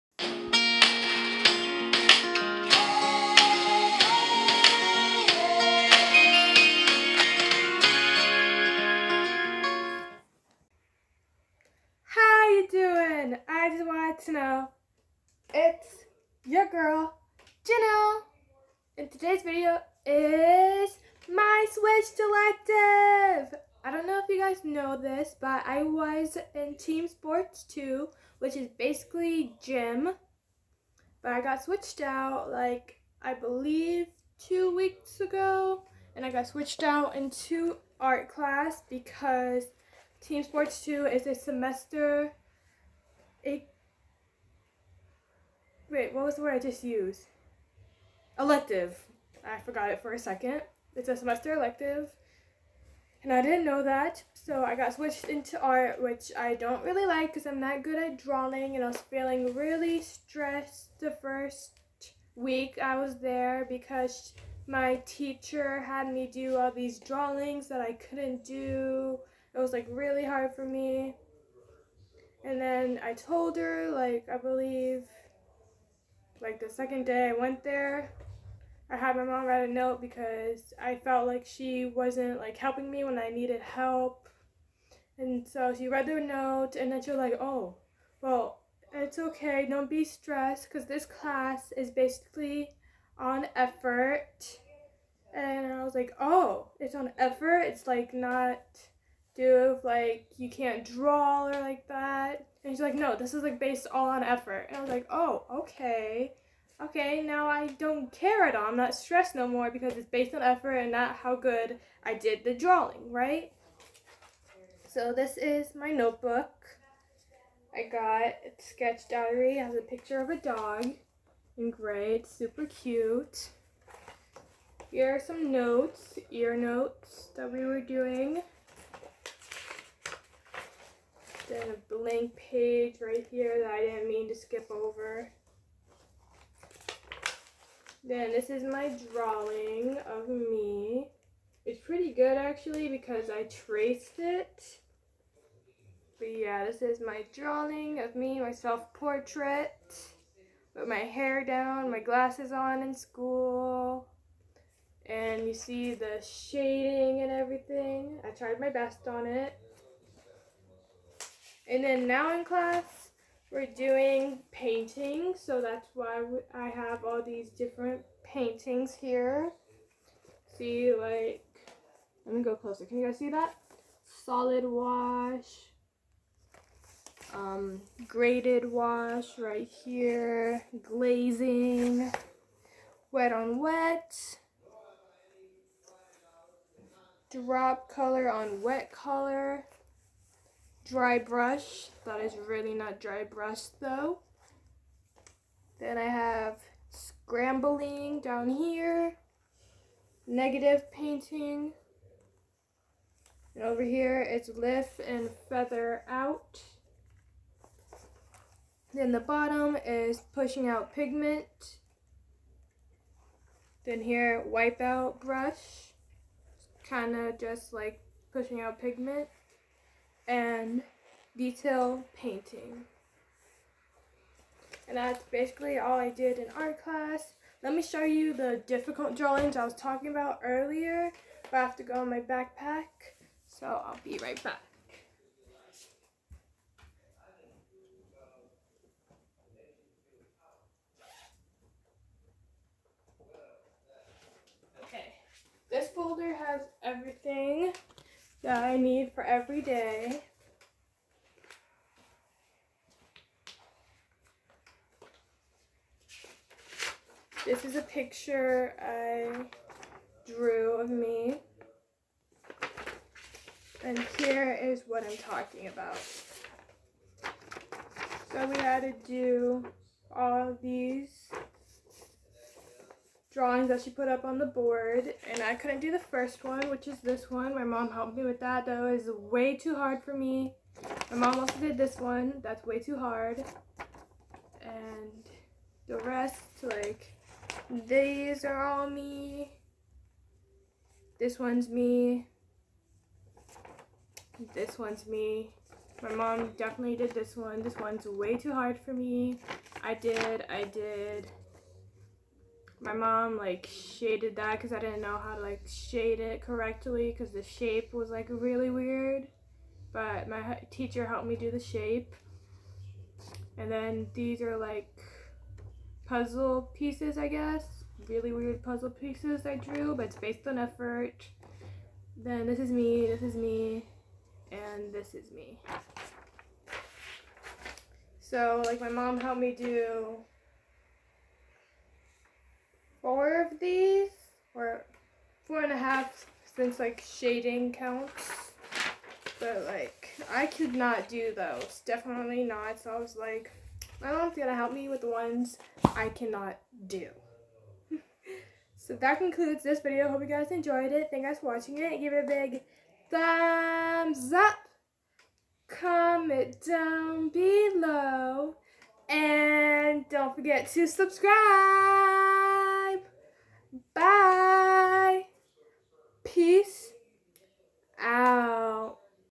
My new elective.
I know you guys most likely will not care about this video, I just wanted to give you guys an update on my schedule.